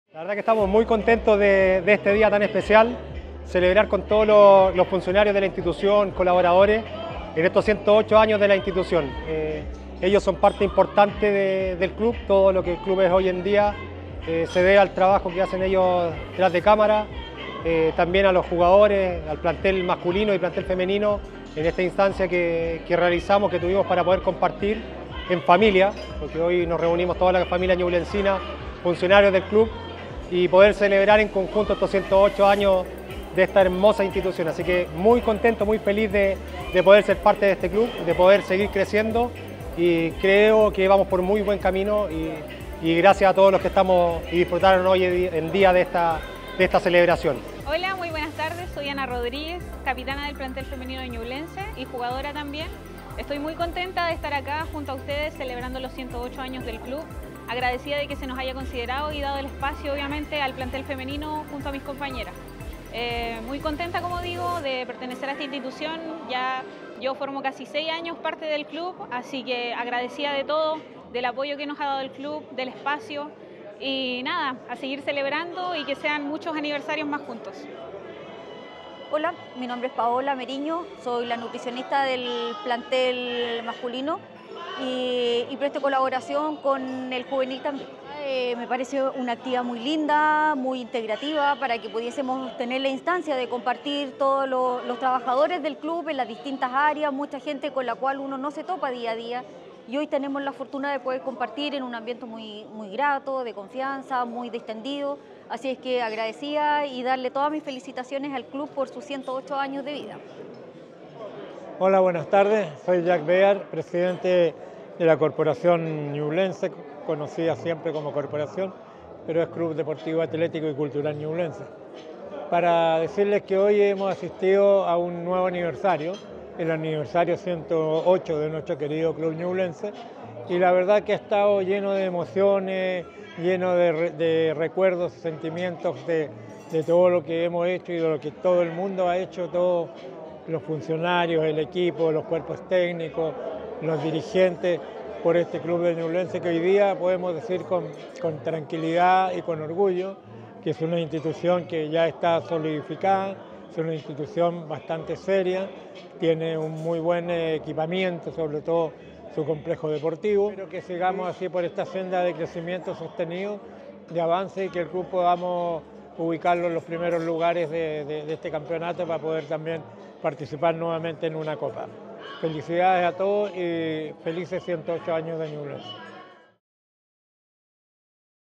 Aniversario 108 • Deportivo Ñublense
En el 108 aniversario de los Diablos Rojos; quisimos celebrar con nuestros funcionarios, cuerpos técnicos, planteles femenino y masculino, dirigentes y colaboradores con un almuerzo de camaradería en el Gran Hotel Isabel Riquelme 👹

En esta oportunidad aprovechamos de reconocer a todos quienes hacen posible el crecimiento de nuestra institución, donde destacamos a los funcionarios con más de 5 años de antigüedad en el club y a tres destacados colaboradores del último tiempo 👏🏻

¡Estamos muy felices de la gran familia que hemos conformado durante este tiempo y esperamos seguir creciendo todos juntos! 🥹

¡FELICES 108 AÑOS, AMADO ÑUBLENSE! ❤️🖤